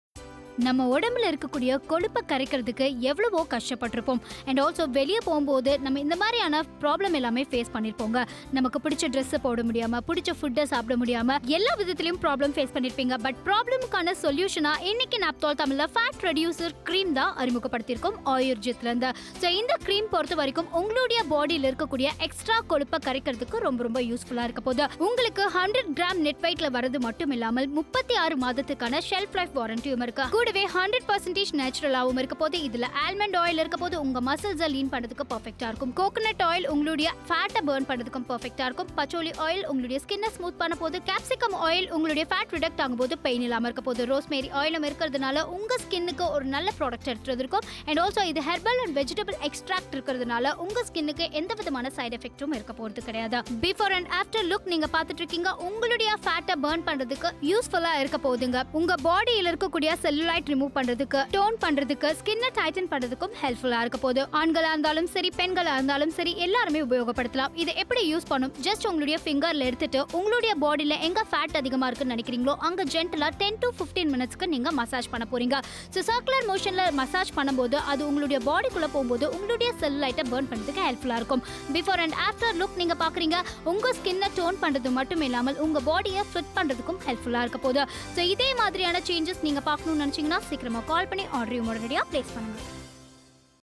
Ayurjeet Fat Reducer Cream (Code:11388)
From fat to fit

Finding it difficult to reduce the cellulite from your body? Look nowhere else, Ayurjeet Fat Reducer Cream is a perfect solution that is developed from natural essential oils which help to fight and melt the cellulite in all parts of your body. This cream also helps regenerate the skin and give strength to the connective tissues that are responsible for tightness, elasticity, firmness and suppleness to the skin. In this offer you get 2 for the price of 1.

Highlights:
Help to reduce fat
Tightens and tones skin
Remove cellulite, tone & tighten the skin
Non-greasy cream
Goodness of herbal & vegetable extracts
Get a firm, smooth & toned body
Most effective when used with a controlled diet and exercise

Benefits:
Rosemary - Anti Inflammatory
Coconut - Fatty Acid Burns Fat
Almond Oil - Development of Lean Muscle Mass
Patchouli Oil - Smoothen skin
Capsicum Oil - Contain Neuropeptide which helps to relieve pain while fat reduction

Features:
Type: Fat Reducer Cream
Ingredients: Rosemary, Coconut, Almond Oil, Patchouli Oil, Capsicum Oil
Ideal For: Men & women
Expiry: 36 Months shelf life
Net Wt: 2 x 100g

Package Contents:
2 x Ayurjeet Fat Reducer Cream (100g)

Country of Origin:
India